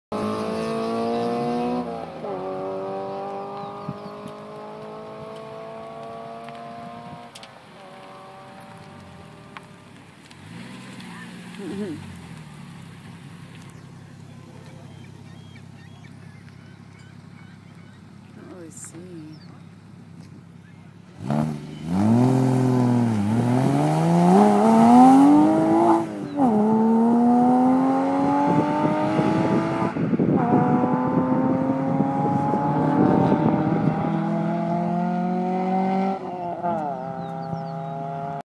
Porsche Cayman RS Take off custom headers and exhaust Boxst
Taking off down front straight in Cayman RS with custom ceramic coated headers and exhaust with primary cat delete and 200 cell high flow secondary cats.